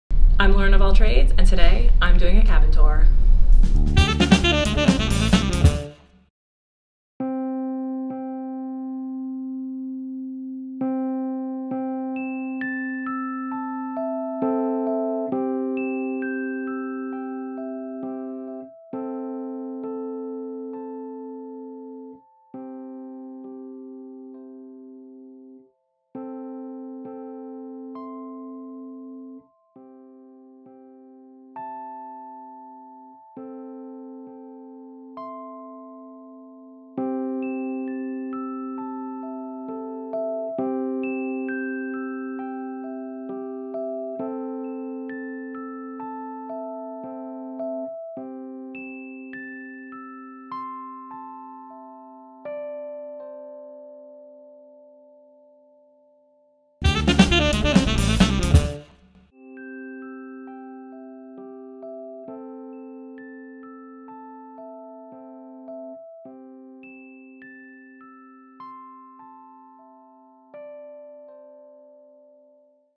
Extended Balcony Cabin 15338 Tour... Carnival Mardi Gras Cabin
In this episode of Lauren of All Trades... Lauren tours Extended Balcony Cabin 15338.

This particular Extended Balcony is category 8K. Extended Balconies have the same interior space and features as standard balcony cabins, but with a deeper balcony space. The extended balcony sizes vary by location, so you'll want to refer to the deck plan to choose the best one!